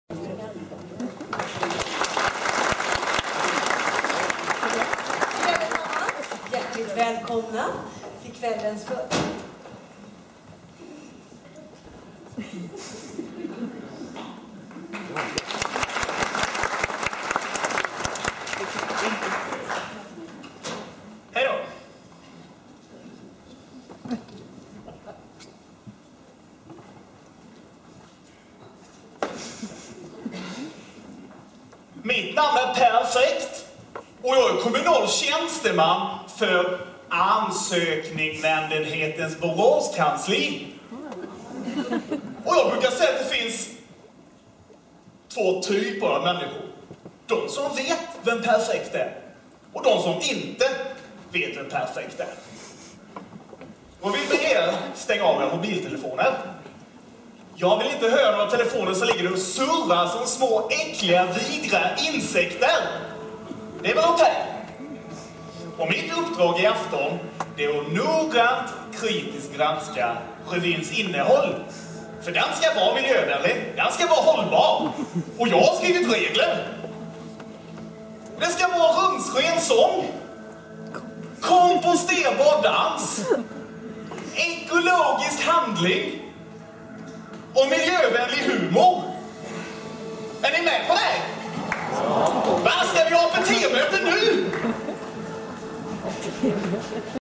Christian Haglund som Per Fäkt
Christian Haglund som Per Fäkt, nyårsrevy i Borås 2015.